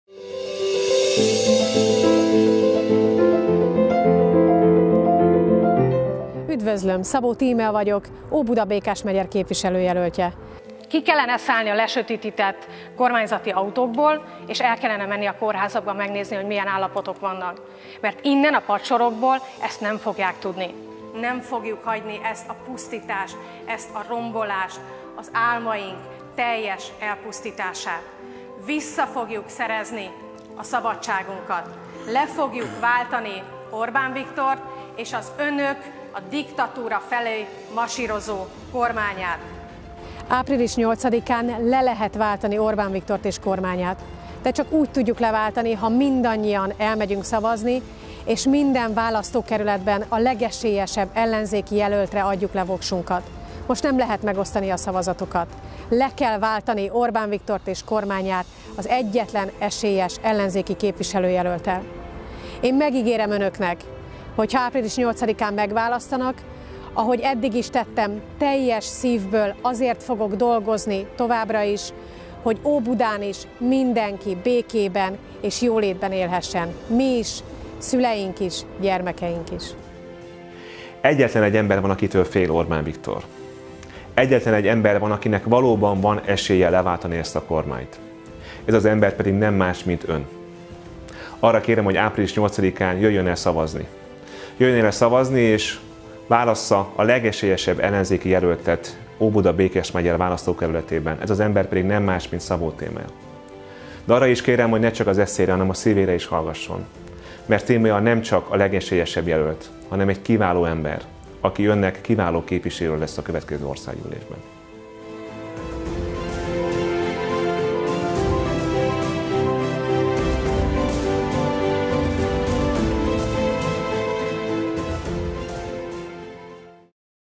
Óbudán legyen Karácsony és Szabó!
Csak úgy győzzük le Orbánékat, ha mindannyian elmegyünk szavazni, és minden körzetben a legesélyesebb jelöltre szavazunk!

A Medián friss kutatása is megerősítette a korábbi méréseket, hogy Óbuda-Békásmegyeren Szabó Tímea (MSZP-Párbeszéd) a legesélyesebb ellenzéki jelölt. A küzdelem szoros, ezért minden szavazatra szükség van!

Győzzük le közösen Orbánt, győzzük le közösen a kerület fideszes jelöltjét, Menczer Erzsébetet, hogy utána közösen építhessünk egy olyan Magyarországot, ahol a hatalom nem önmagát, hanem a magyar embereket gazdagítja! 👉2 szavazat = kormányváltás. Ha változást akarsz, szavazz a MSZP-Párbeszéd listájára és a legesélyesebb ellenzéki jelöltre! ✘ MSZP-Párbeszéd ✘ Szabó Tímea